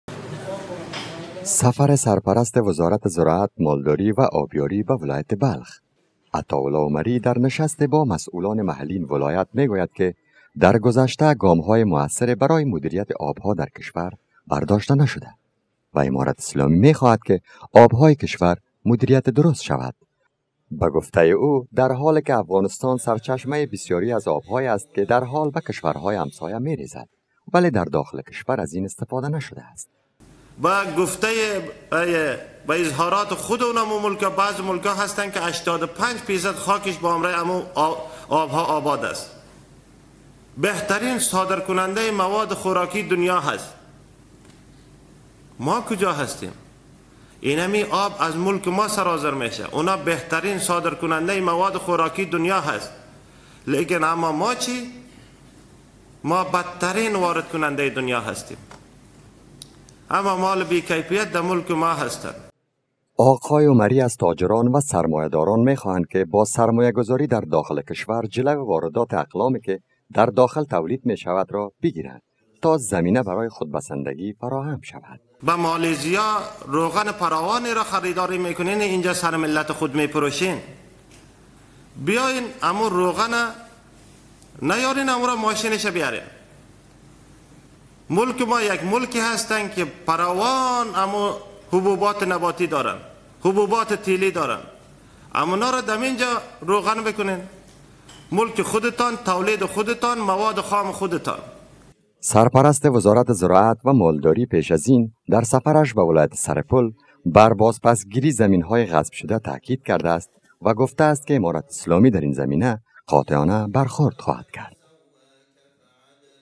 Minister criticizes failure to control waters in the past | انتقاد عمری از مهار نشدن آب‌های کشور
Minister criticizes failure to control waters in the past
انتقاد عمری از مهار نشدن آب‌های کشور